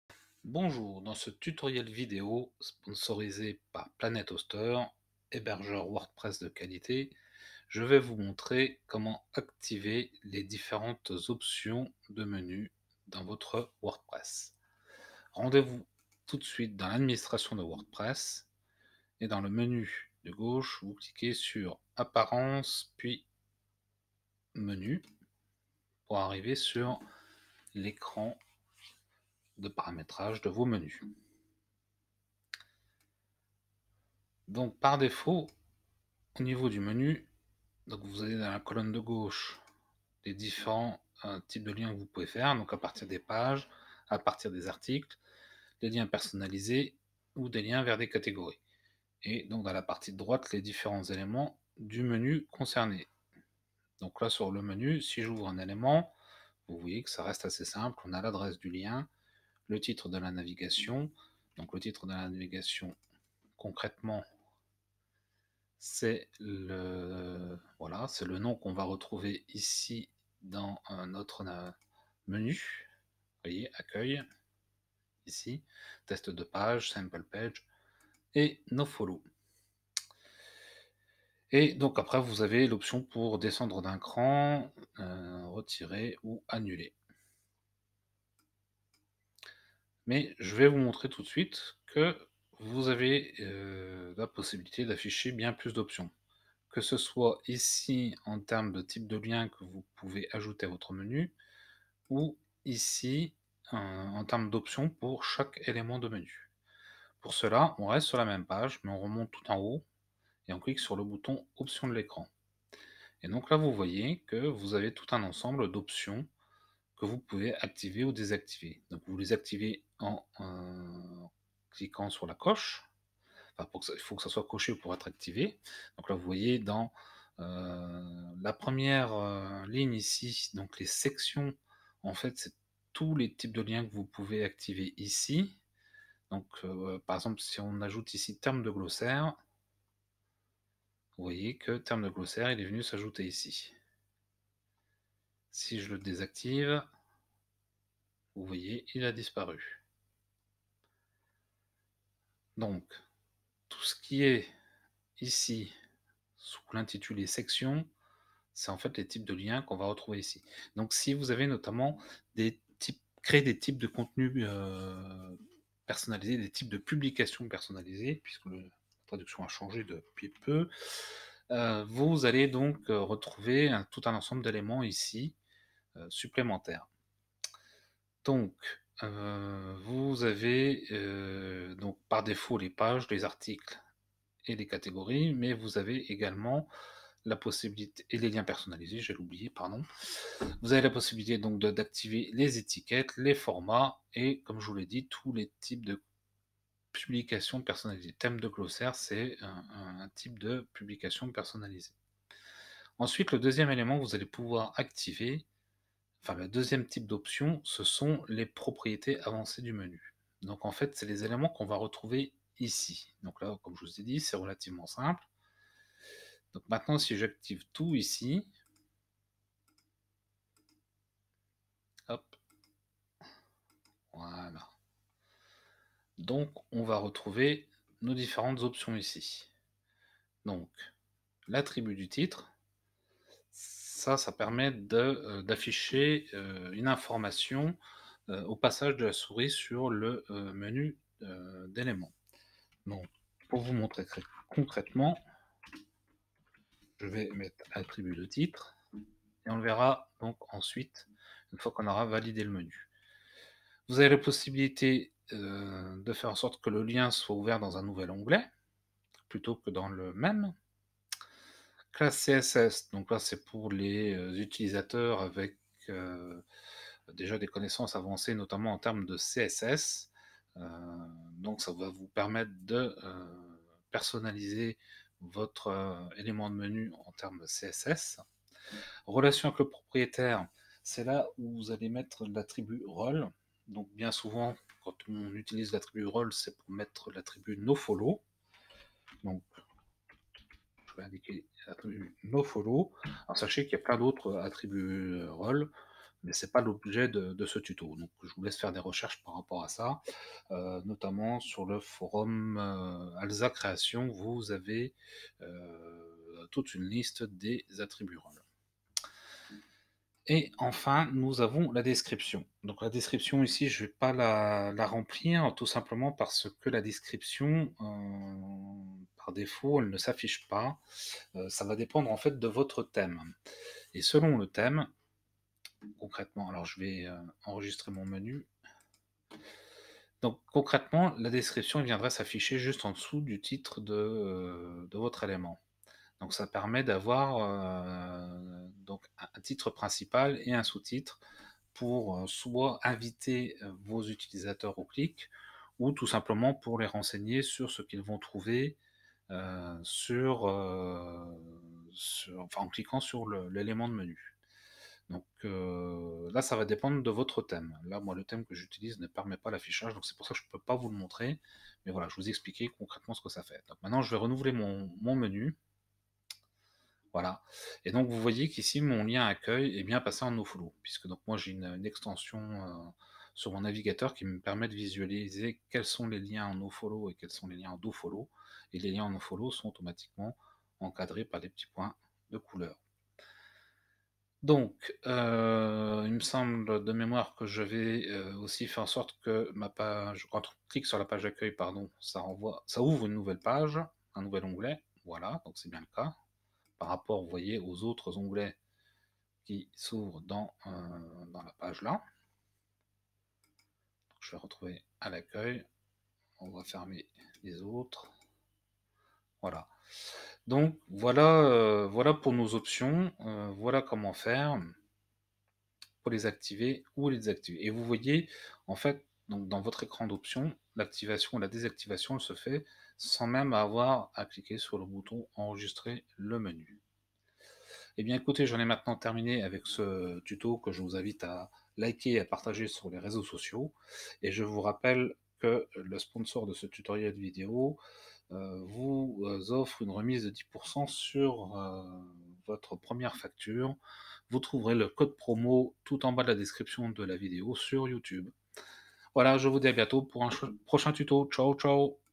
WordPress : comment activer l’affichage des différentes options de menu
vous montre comment activer l’affichage des différentes options de menu sur WordPress afin de personnaliser les différents éléments de votre menu.